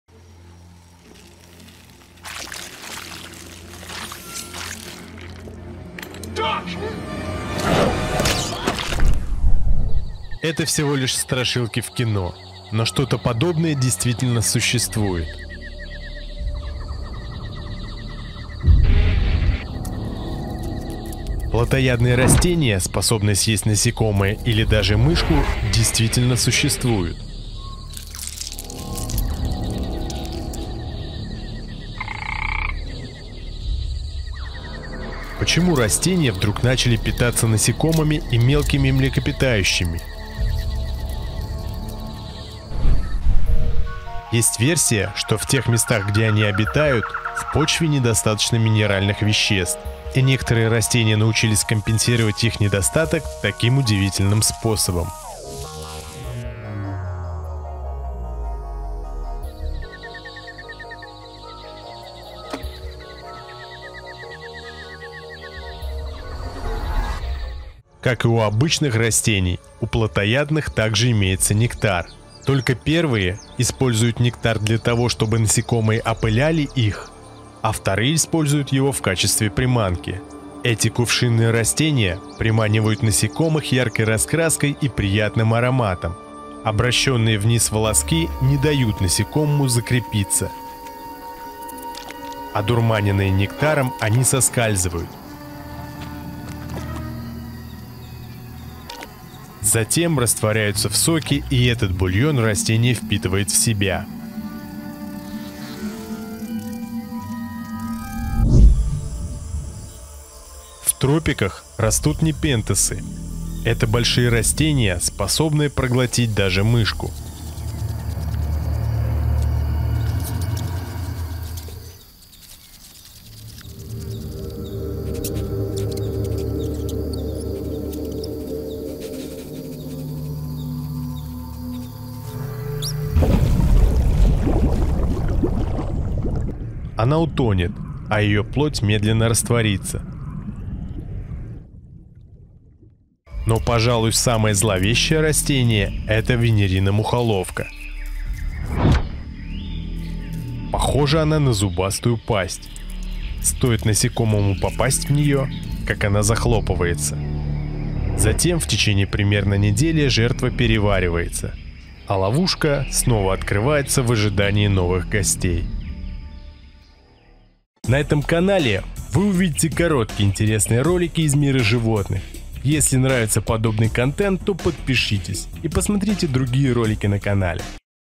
Растения которые любят мясо. Хищные растения!
Плотоядные растения способные съесть насекомое или даже мышку действительно существуют.
Почему растения вдруг начали питаться насекомыми и мелкими млекопитающими. Есть версия, что в тех местах где они обитают, в почве недостаточно минеральных веществ и некоторые растения научились компенсировать их недостаток таким удивительным способом.